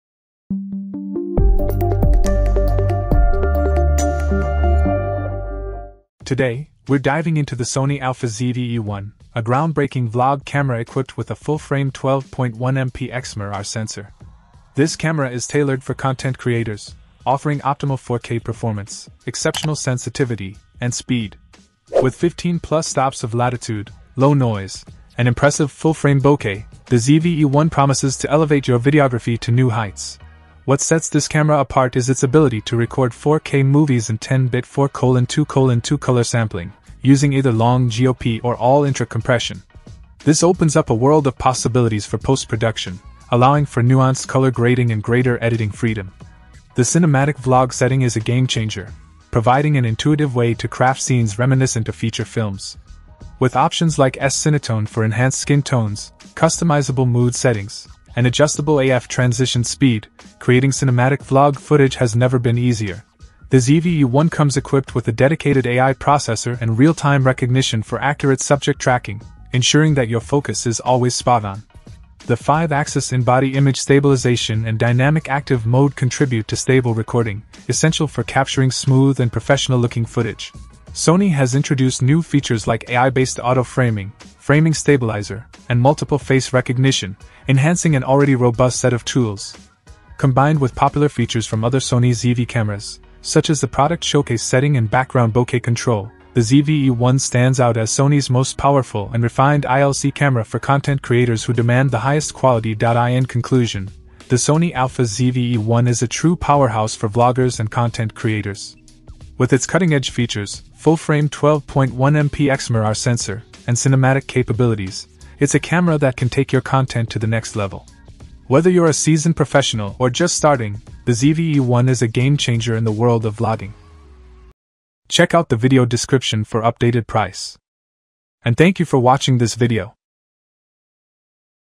Sony Alpha ZV-E1 Review: A Game-Changer for Vloggers?
0:00 Intro
0:06 Review

Things that we mentioned in this video:
Sony Alpha ZV-E1 Full-Frame : B0BZWNWCZ9